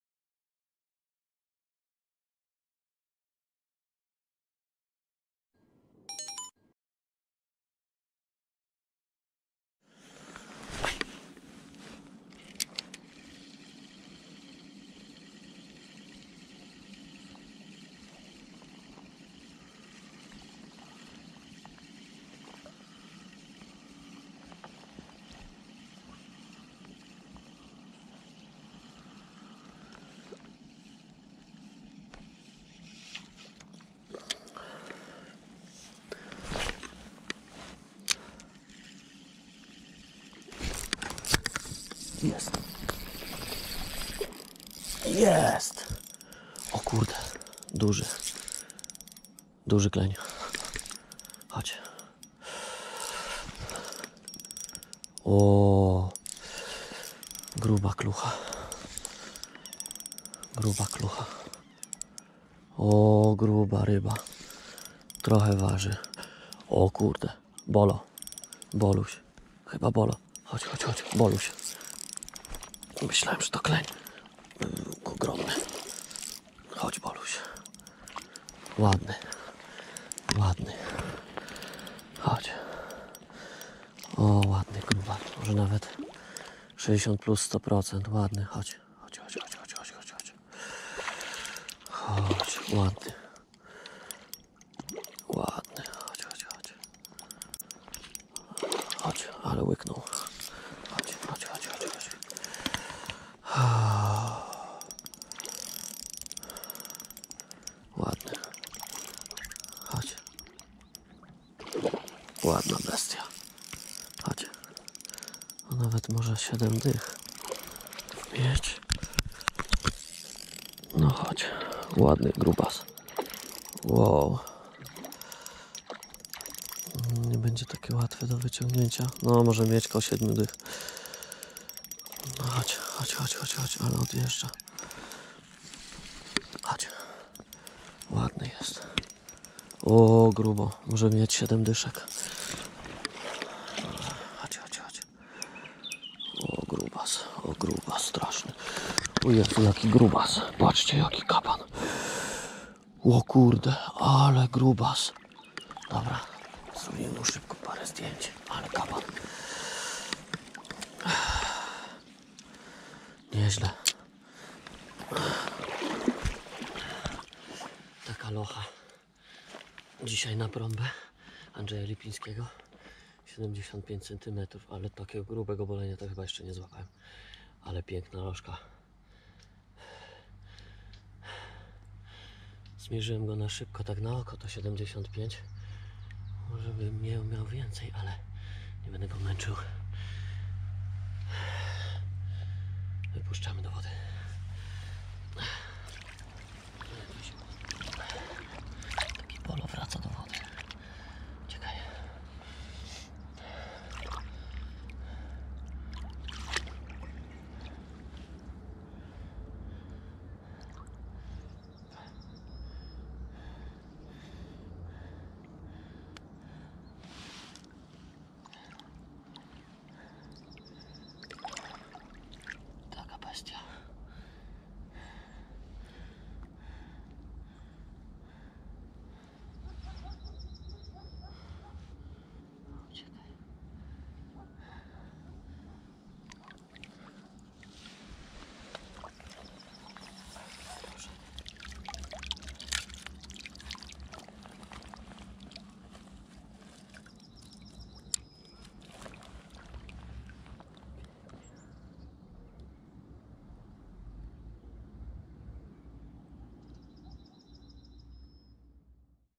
Potężny Boleń z Sanu na Brombę Andrzeja Lipińskiego | Asp Fishing
Piękny Boleń 75 cm złapany na największą Brombę Andreja Lipinskiego z Sanu :)

Sprzęt na Filmie :
Wędka - Batson RX6 2-10 g 290 cm
Kołowrotek - Shimano Shepia 3000S
Żyłka- Dragon HM 80 0,16mm
Przynęta- Bromba A. Lipińskiego
Kamera - Go Pro 7 Black

Zapraszam do subskrypcji mojego kanału żeby być na bieżąco :)
będę wdzięczny za udostępnienia filmu:)